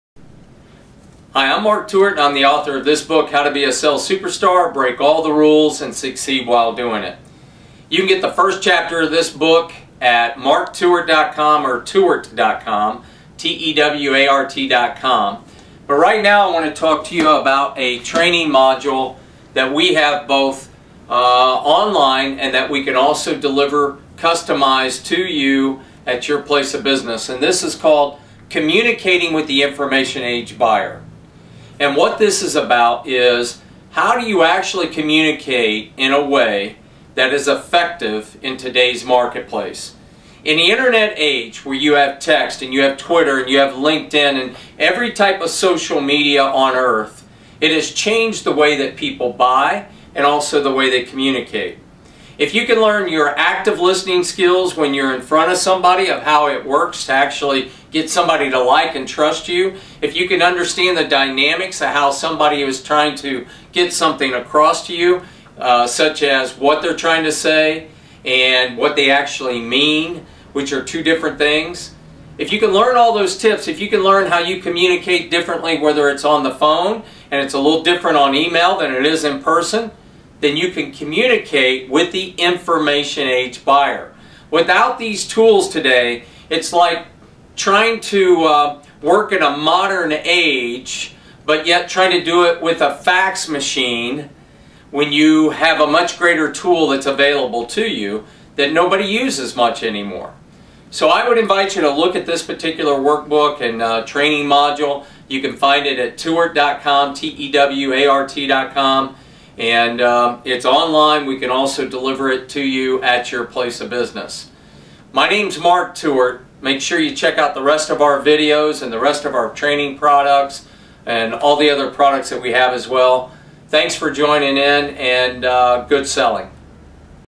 Automotive Sales Training - Communicating With The Information Age Buyer by Mark Tewart
Automotive Sales Training Expert Mark Tewart discusses his training module "Communicating with the Information Age Buyer." Discover how to communicate with your customers effectively in the digital age. Understand the Do's and Don'ts of communicating to customers. For additional information and to get the 1st chapter of "How To Be A Sales Superstar" for FREE go